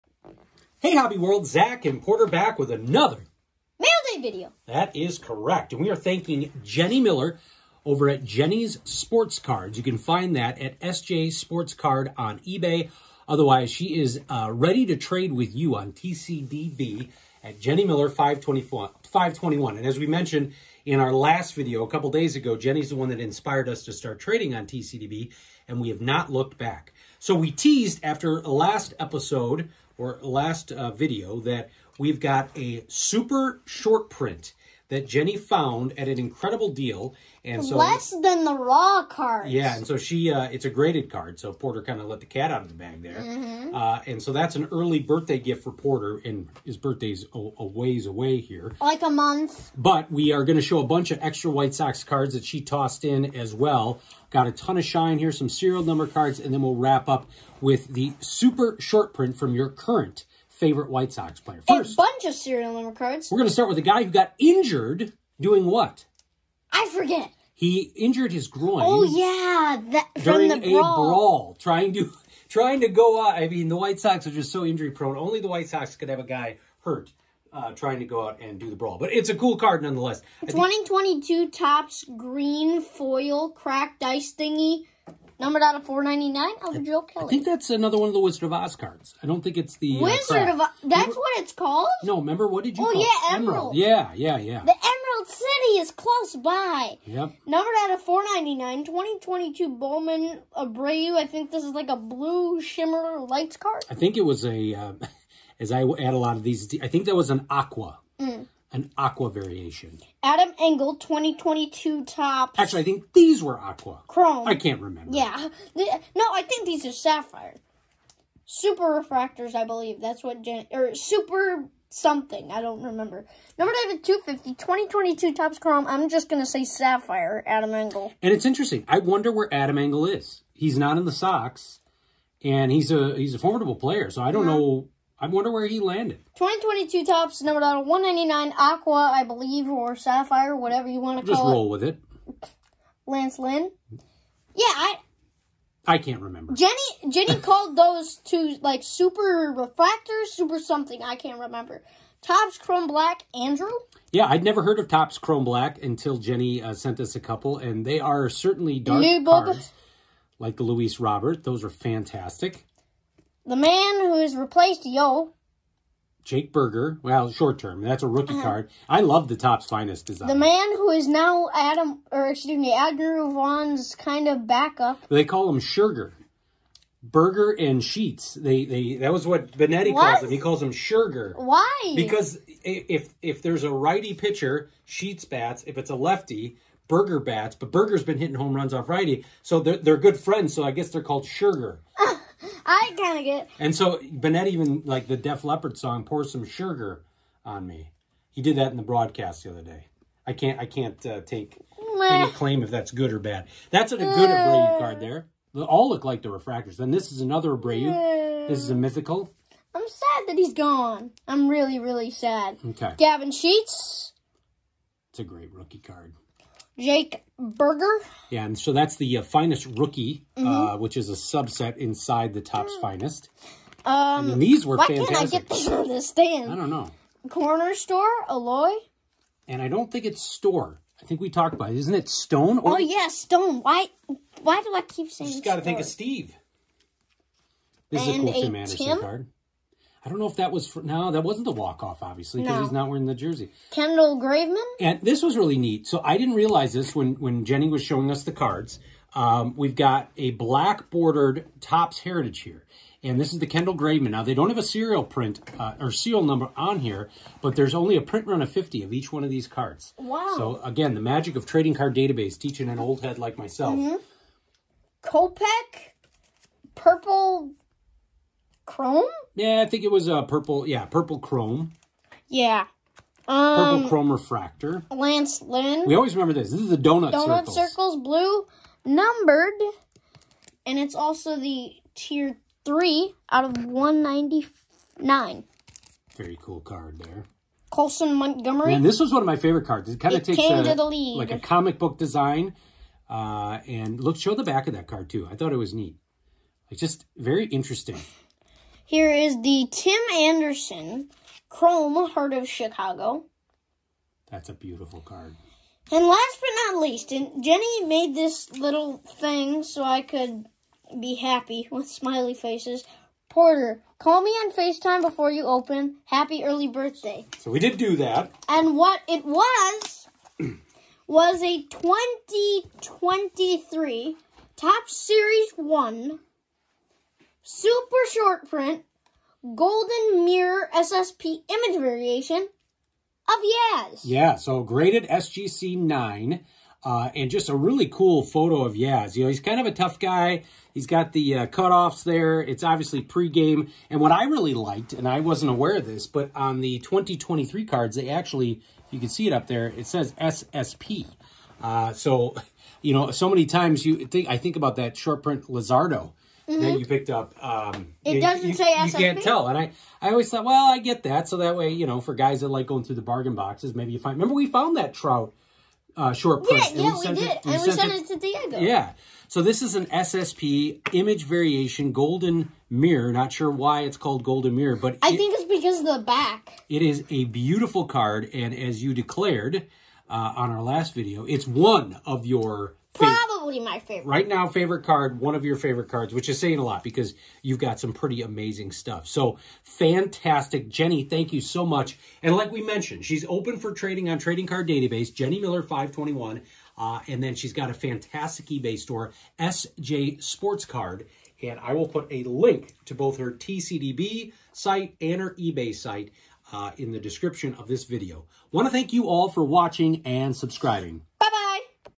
Mail Day - Thank you @ValueBoxDigger Part 2 - 2023 Topps SSP!
Thank you @jennymiller8653 for an amazing whitesox mailday. Today is Part 2 sharing some great cards we picked up from Jenny, including a 2023 Topps SSP Jenny sent as an early birthday gift for Porter. Amazing stuff!

You can find Jenny on TCDB, Twitter and EBAY.